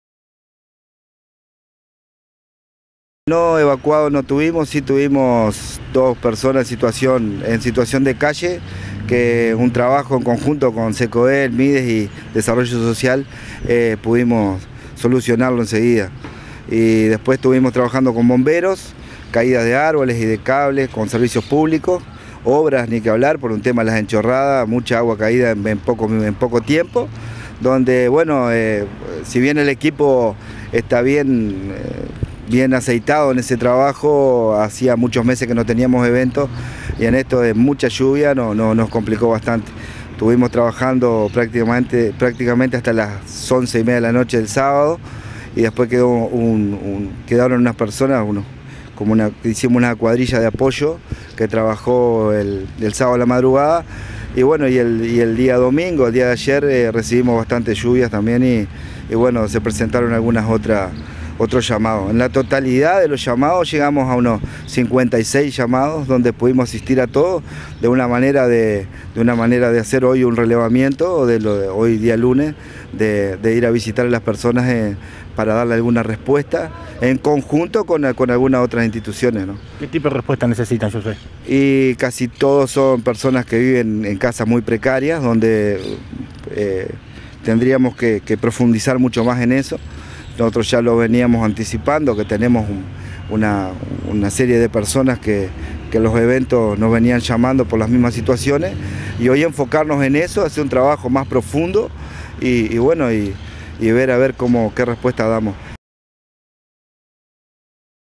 Tormenta: comité de emergencias asistió a 56 llamados de ayuda
El coordinador del CECOED, Josué Lima, dijo que más de 50 personas fueron afectadas en forma directa y otras tantas en forma indirecta por la tormenta de lluvia y granizo, sobre todo en la zona Noroeste de la ciudad que fue la más afectada. Hasta las 10 de la mañana del sábado se habían recibido alrededor de 30 llamadas -la mayoría por enchorradas y rotura de techos- y se estuvo trabajando con apoyo de equipos de Obras, Servicios Públicos y Bomberos.